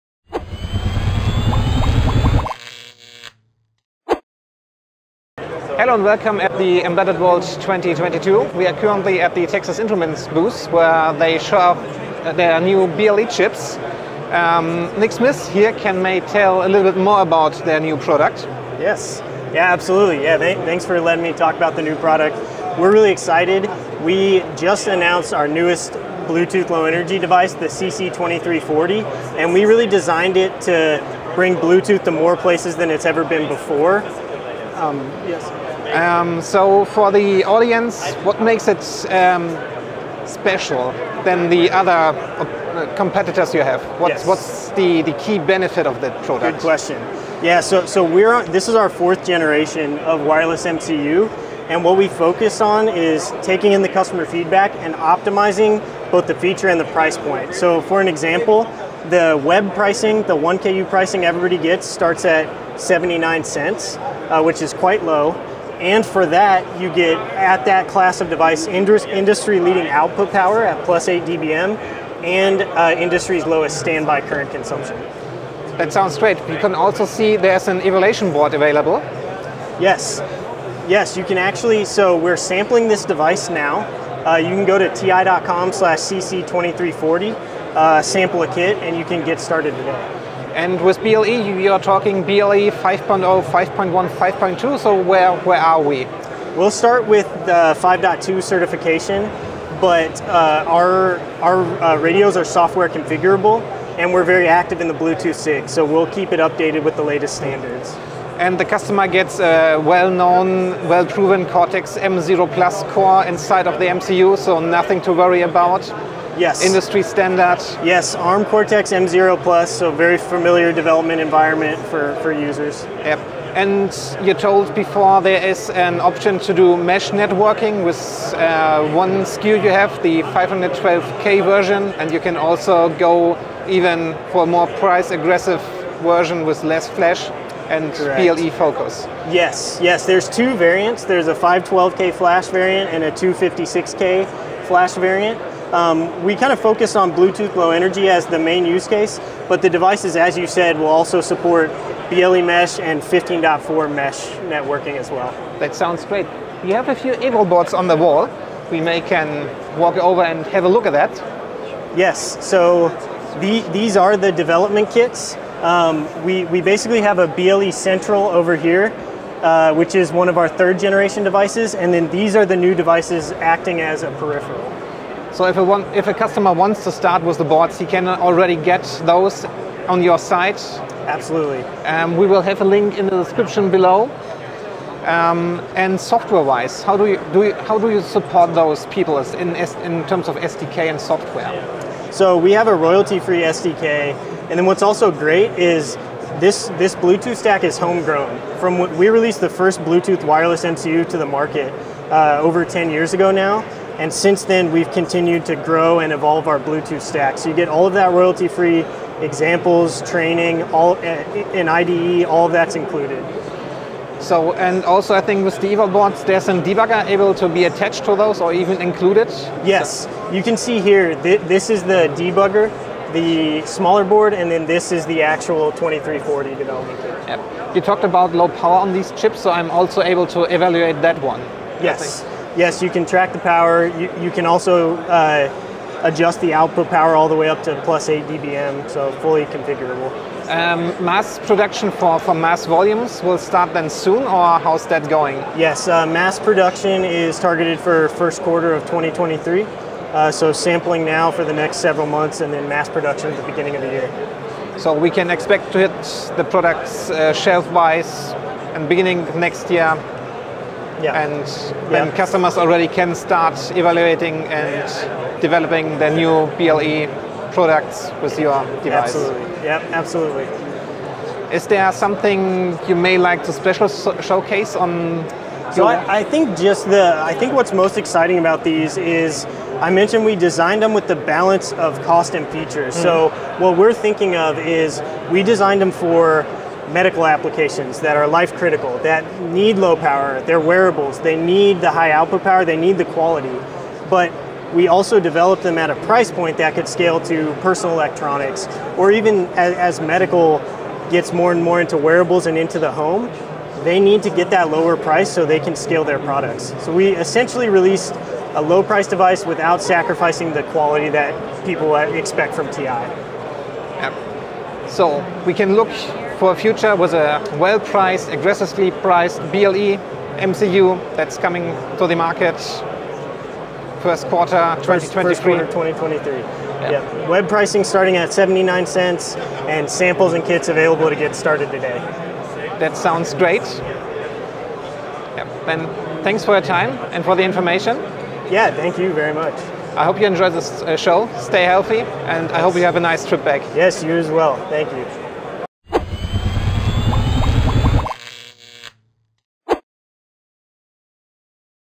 Elektor Checks Out the Texas Instruments CC2340 at Embedded World 2022
Texas Instruments announced a new Bluetooth LE device at Embedded World 2022. Elektor engineer Mathias Claussen got the details from TI engineer Nick Smith about the SimpleLink Bluetooth LE CC2340 wireless MCU, as well as details about currently available boards.